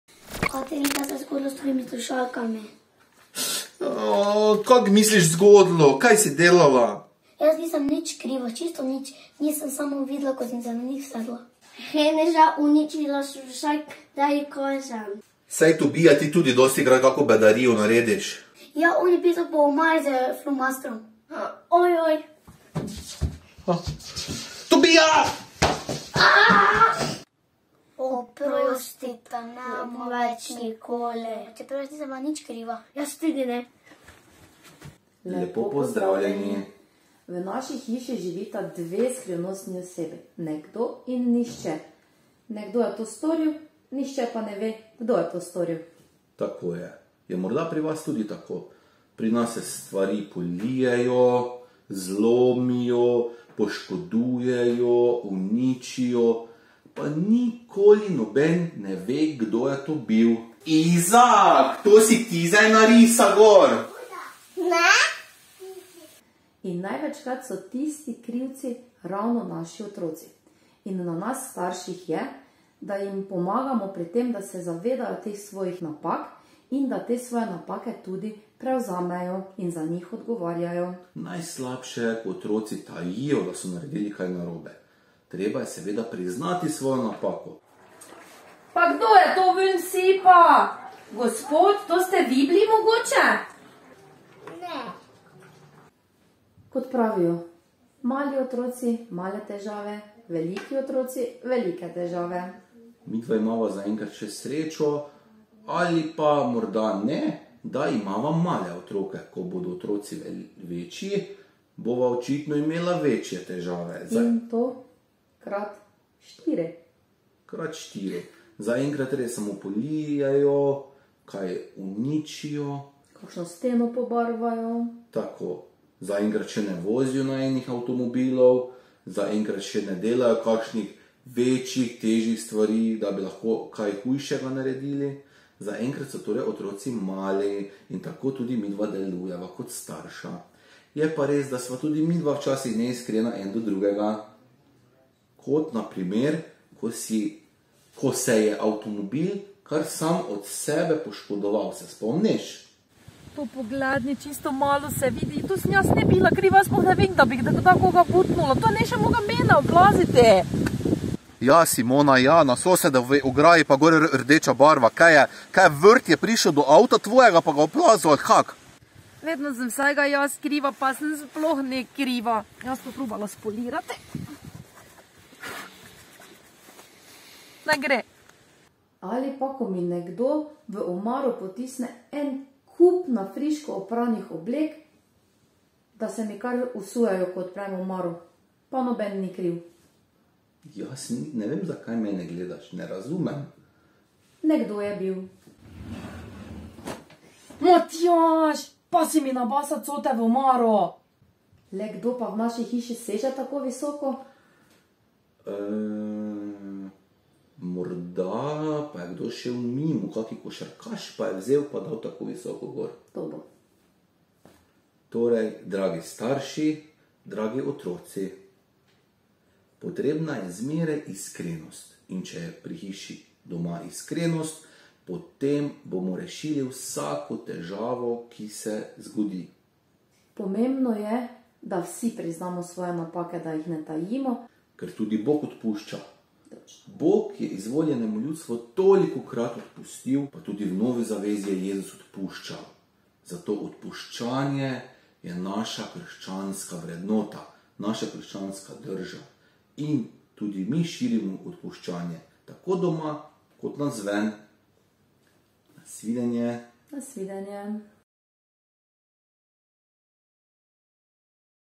Zakonca Kosi, vlog #4: skrivnostni osebi v družini
Pri Kosijevih poleg štirih otrok ter Simone in Matjaža živita še dve skrivnostni osebi. Veste, kdo sta? Zakonca Kosi vam v vlogu razkrijeta tudi, kakšen naj bi bil krščanski pogled na vzgojo, tudi ko ne gre vse po maslu. Več